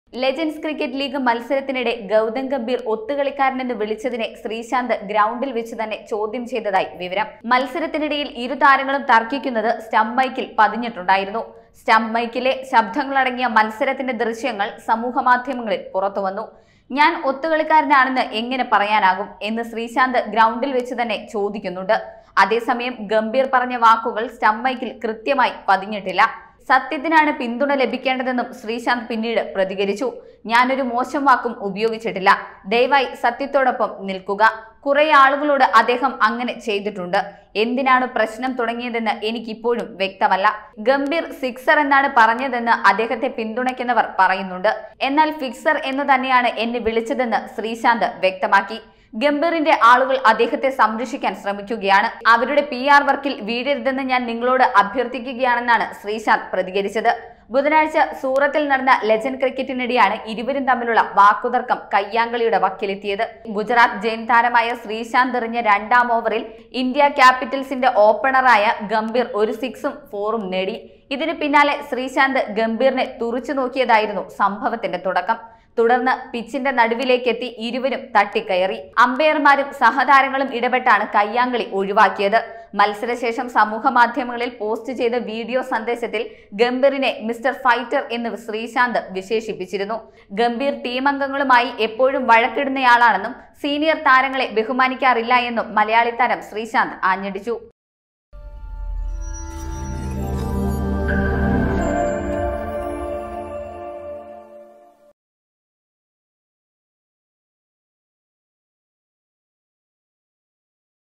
തമ്മിലടിച്ച് ശ്രീശാന്തും ഗംഭീറും . പോര് മുറുകുന്നു
Tag-Generator-Youtube-malayalam news-global